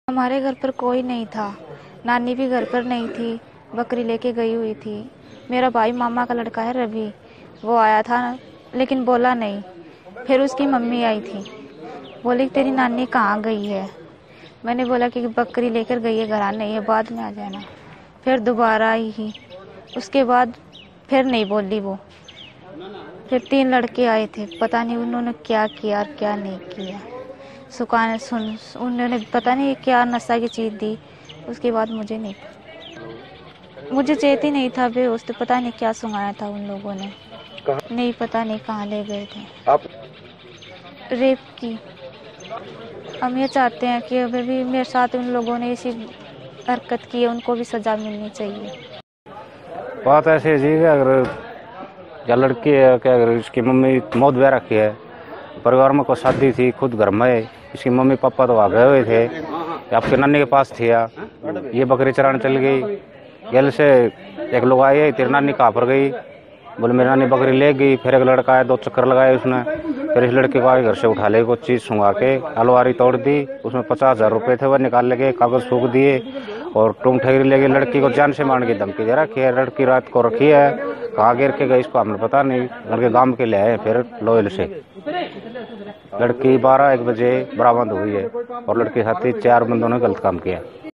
झुंझुनूं में आपसी रंजिश में युवती का अपरहण कर किया दुष्कर्म
झुंझुनूं. चिड़ावा थाना इलाके में एक युवती का अपहरण कर साथ सामूहिक दुष्कर्म करने का मामला सामने आया है. जिसमें पुलिस ने पीड़ित का मेडिकल करवाकर जांच शुरु कर दी है.